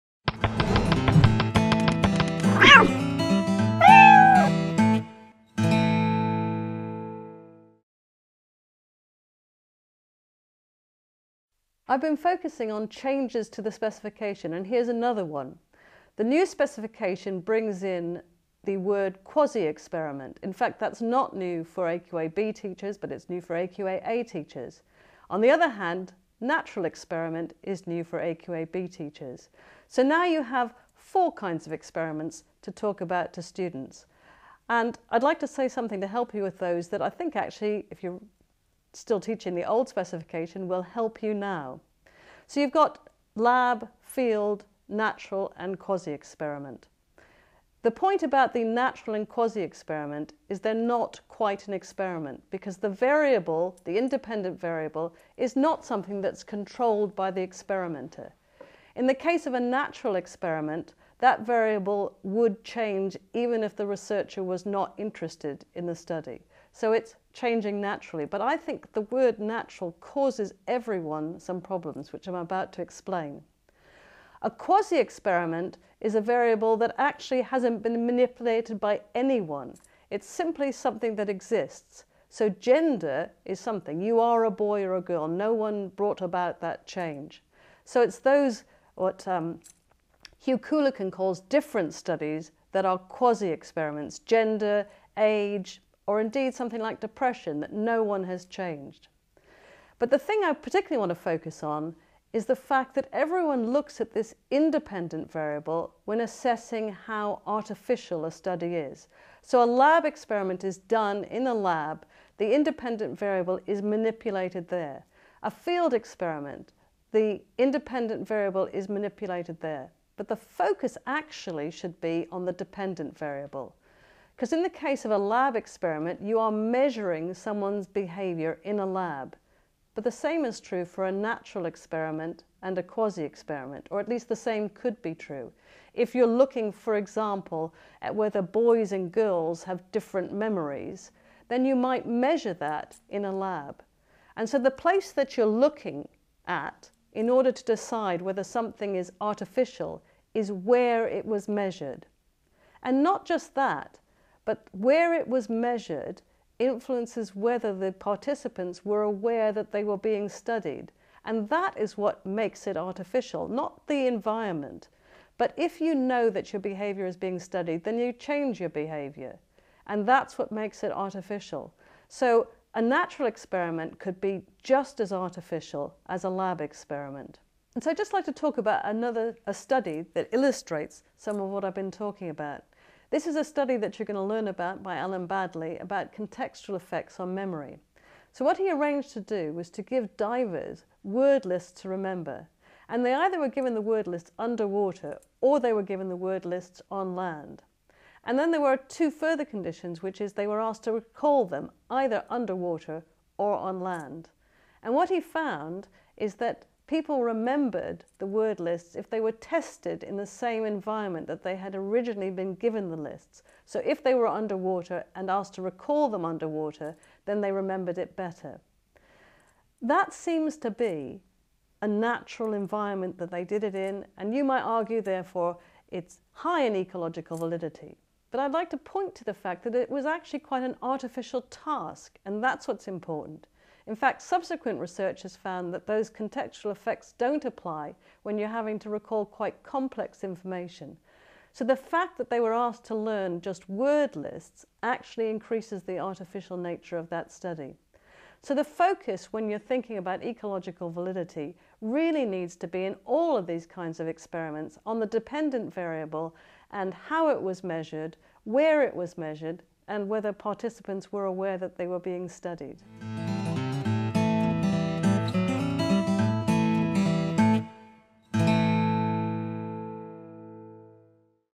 The Complete Companions: Psychological Experiments
Psychological Experiments in the Complete Companions for AQA series. Experienced and trusted co-author of The Complete Companions, Cara Flanagan, discusses the new requirements of the 2015 AQA Psychology AS and A Level specifications, focusing on experiments.